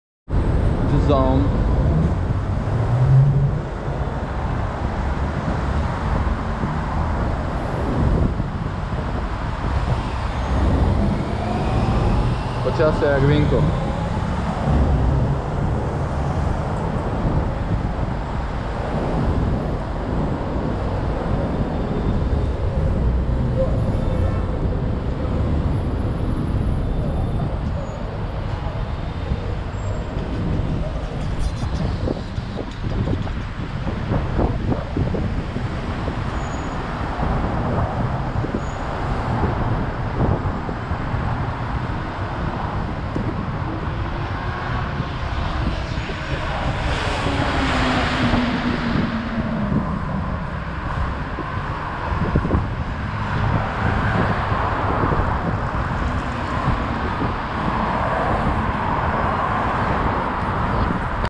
Kotelnicheskaya Embankment Building, Moscow, Russia
walking along the river in Moscow